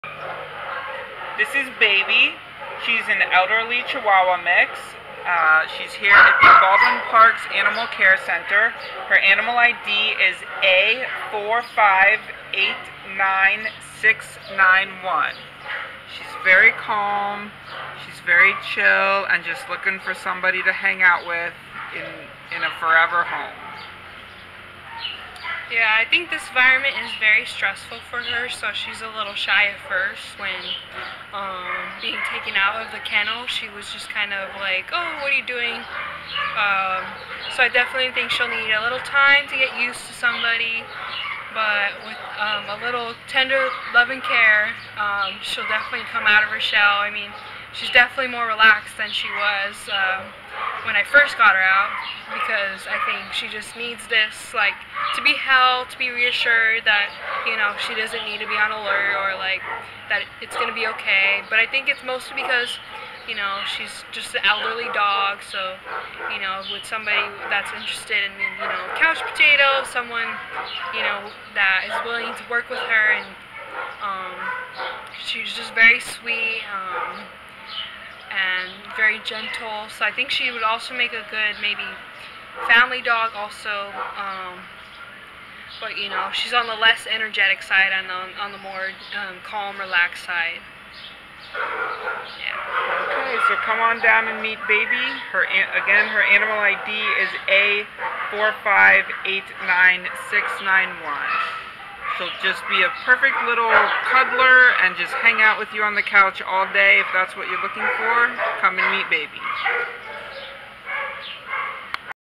A4589691 Baby | Chihuahua/Fox Terrier
A4589691 Baby is a gentle 15-year-old white and black female Chihuahua and Fox Terrier mix who was surrendered by her owner to the Baldwin Park Animal Care Center on May 8th. Weighing 7 ½ lbs, Baby is a tiny, sweet little couch potato who loves to be held and carried. A senior girl who appreciates warmth and cuddling, Baby will get comfortable in your arms and fall asleep for a good nap as the highlight of her day. She has some senior health concerns including mammary tumors and teeth issues that should be addressed by an outside veterinarian, but her heart full of love to share with her angel adopter. She’s a little stressed in the noisy kennel environment and confused about how she ended up here so for now, she likes to have some time to get acquainted with new people. Because she startles easily we think she would be happiest in a quiet home environment with only adults or teens rather than little kids who might not respect her small size and boundaries.